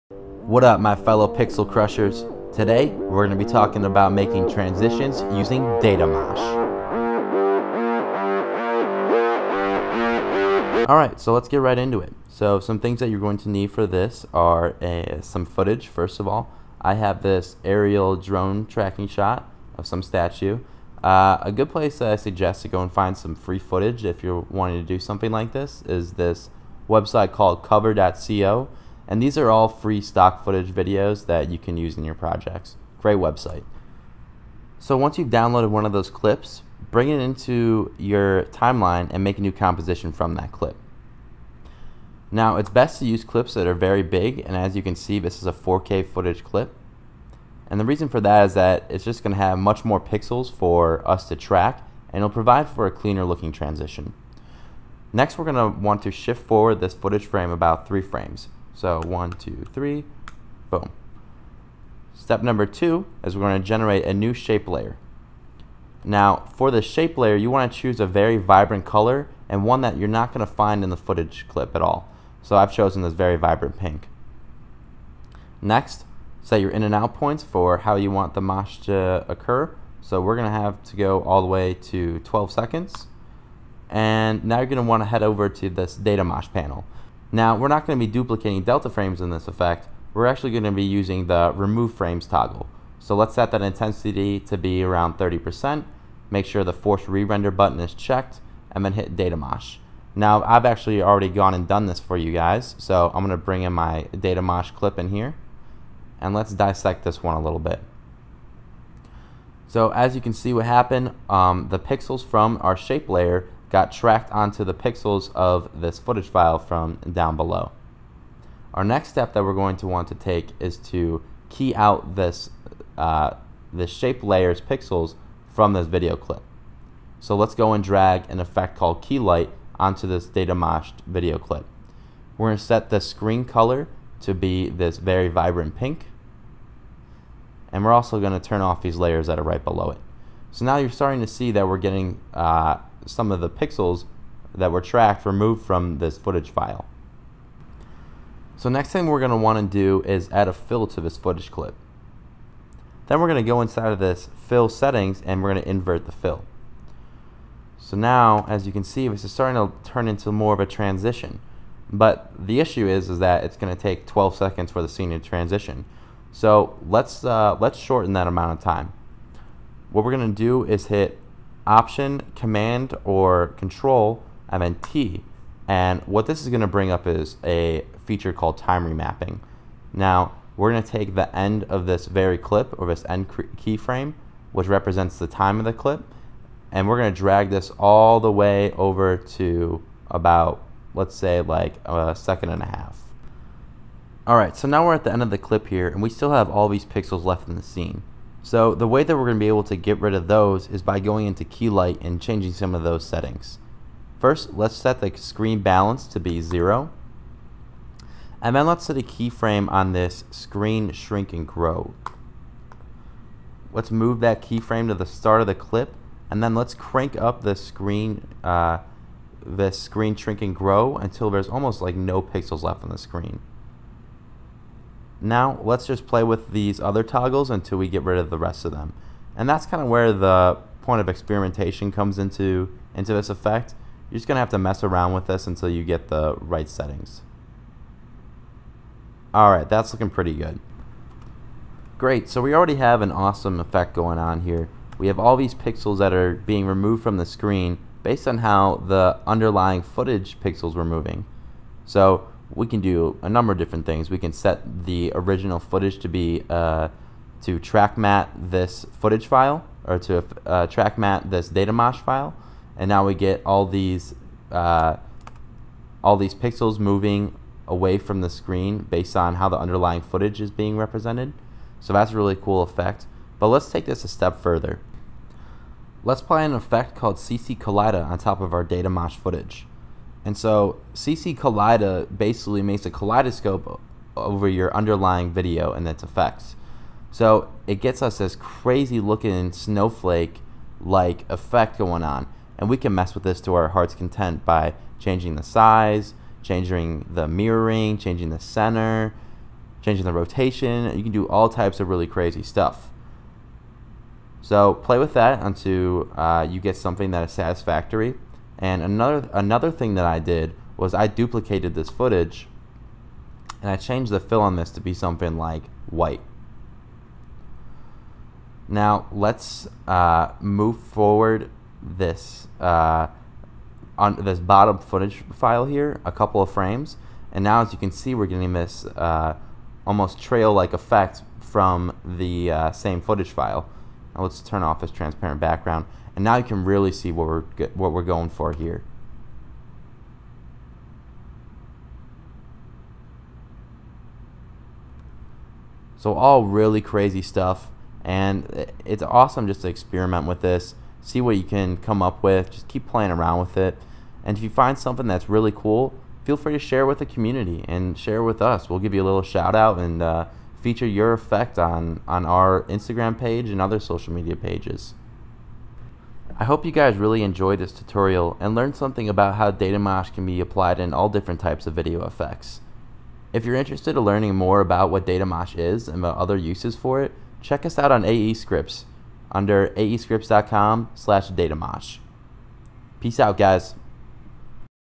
Datamosh quick tip | Making custom transitions with Datamosh
Learn how to make wild transition in After Effects using Datamosh!

We go over the exact steps to create these CRAZY custom transitions. All of the transitions are made by taking advantage of broken media pixels. Get your copy of Datamosh below!